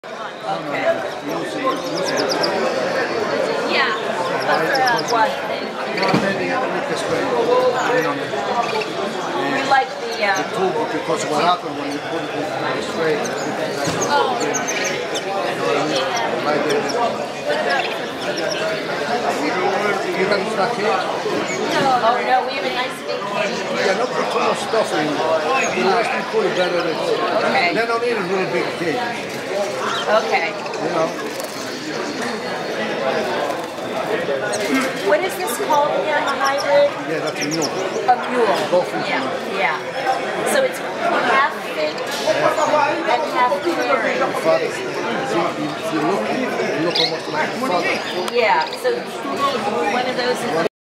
Yellow Canaries - Astoria Bird Club Show 2019
Enjoy the view of some beautiful yellow canaries for sale at the Astoria Bird Club Show 2019